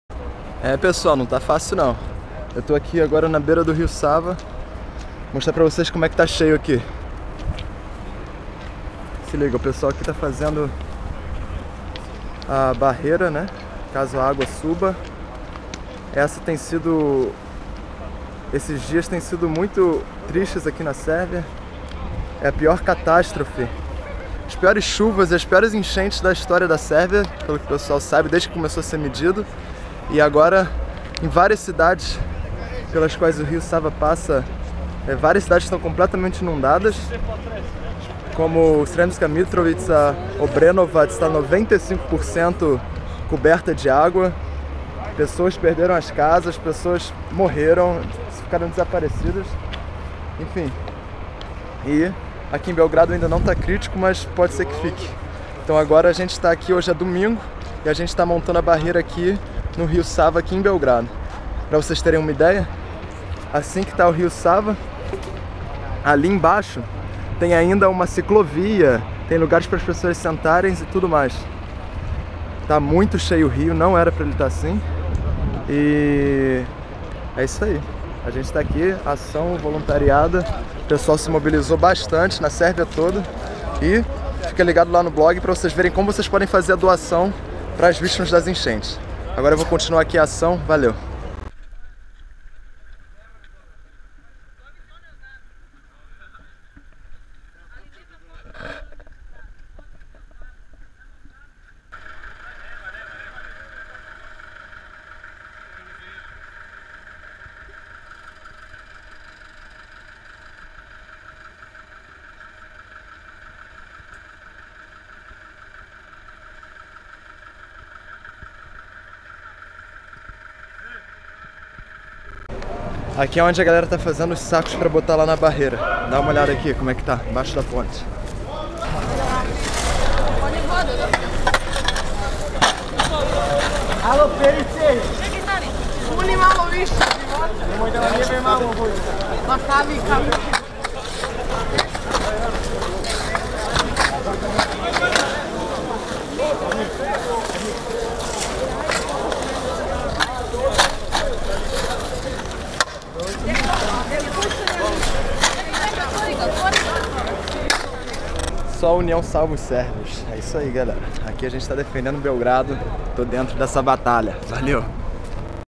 ENCHENTE | Mutirão no rio Sava - Belgrado, Sérvia
A Sérvia está enfrentando a pior enchente da sua história e Belgrado está se preparando para um aumento critíco do nível do Sava! Centenas de cidadãos estão incasavelmente erguendo a barreira com sacos de areia na margem do rio.

Највећа водена катастрофа у историји Србије је у току и Београд се спрема за поплавни талас који стиже! Више стотина грађана упорно пуни џакове песком да се прави насип на обали Саве.

Serbia is facing the worst-ever water disaster in its history and Belgrade is getting ready for the critical increase of the water level on the Sava! Hundreds of citizens have been persistently building a barrier with sand-filled sacks on the riverbank as you can see in the pictures.